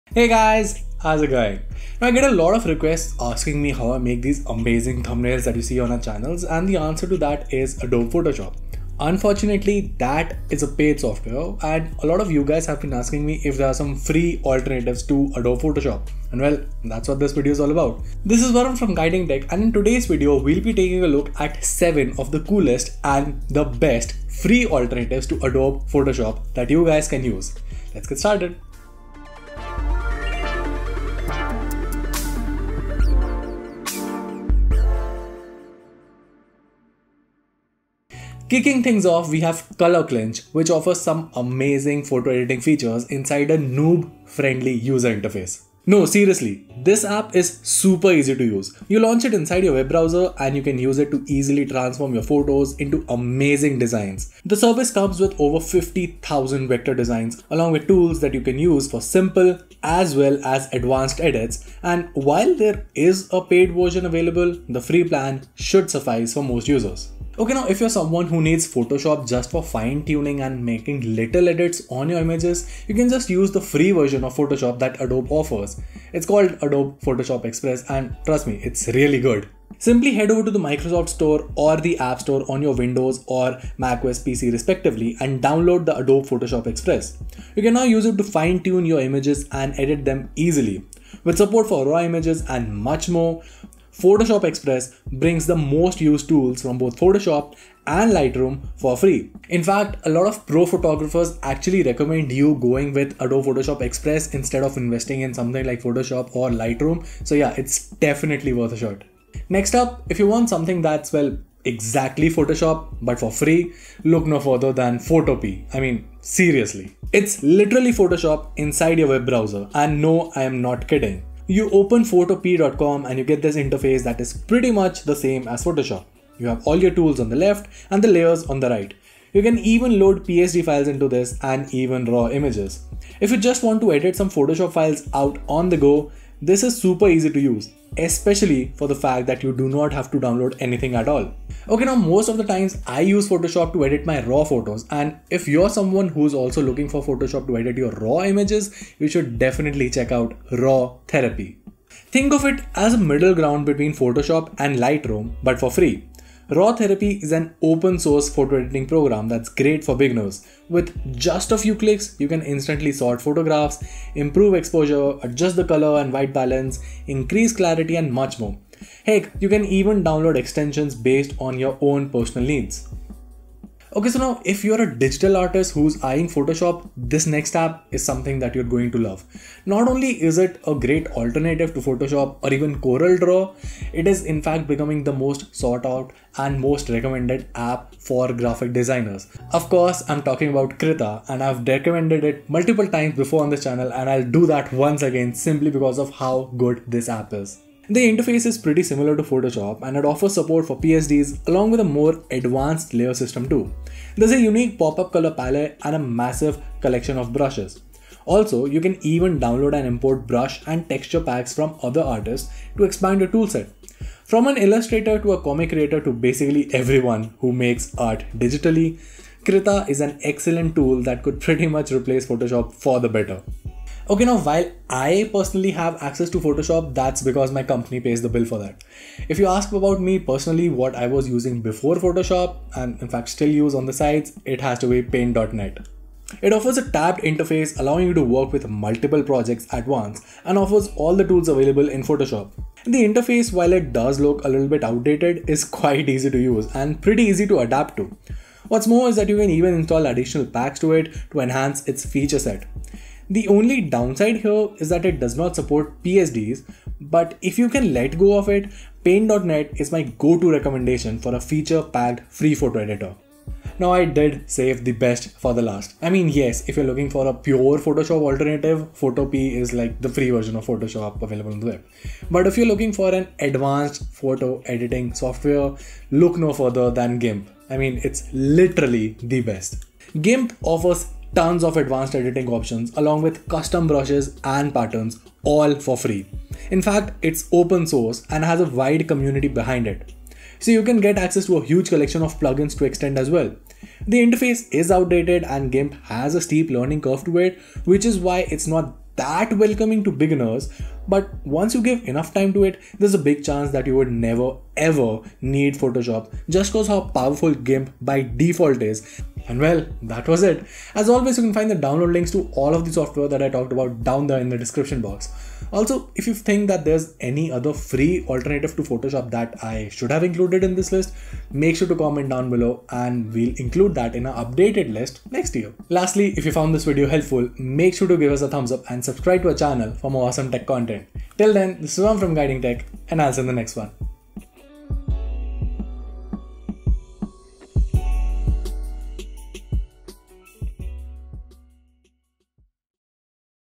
Top 7 FREE Adobe Photoshop Alternatives! | Must Try! | Guiding Tech
Are you looking for a free Photoshop alternative in 2021? Well, today, we will be talking about the top 7 best free Adobe Photoshop alternatives, both online and offline that you should definitely check out!